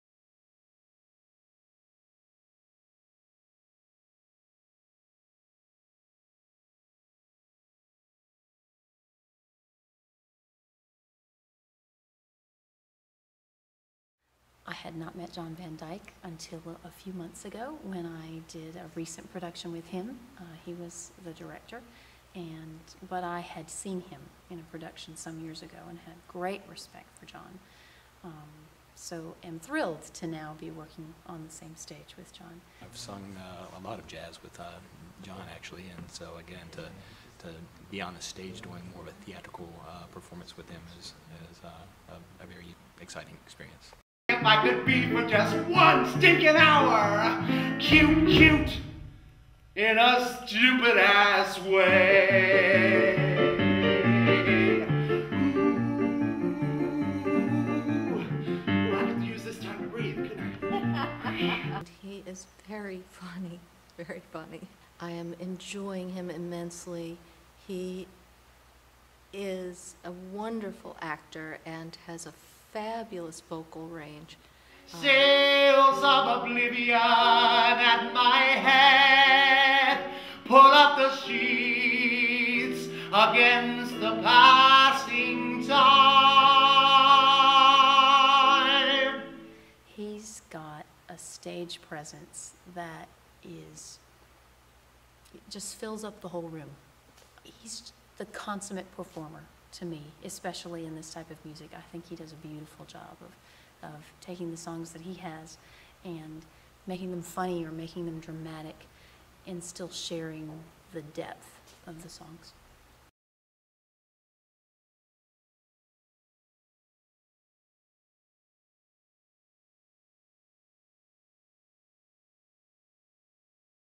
Ratio Theatre Company - Jacques Brel . . . Spotlight on John Van Dyke
Fellow cast Members of Ratio Theatre Company's production of Jacques Brel is Alive and Well and Living in Paris discuss working with John Van Dyke.